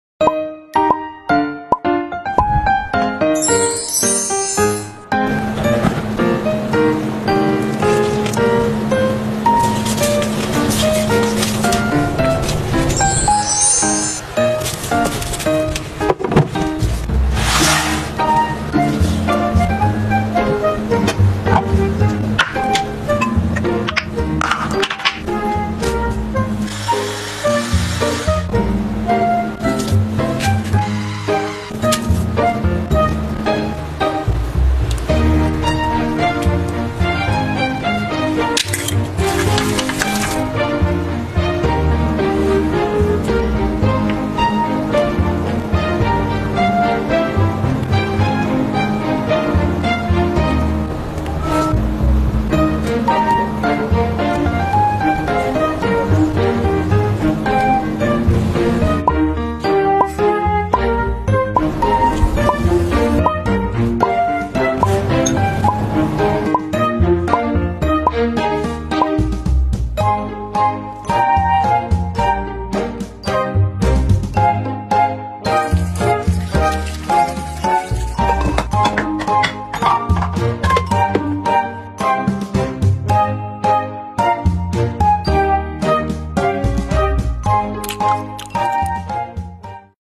🥟🥟把三麗鷗小可愛們做成了餃子~普通の餃子に飽きたら、可愛いサンリオキャラクターの餃子を作ってみよう💜💀👑庫洛米大人 👑💀💜
💜 💀 我們都愛庫洛米大人💀 💜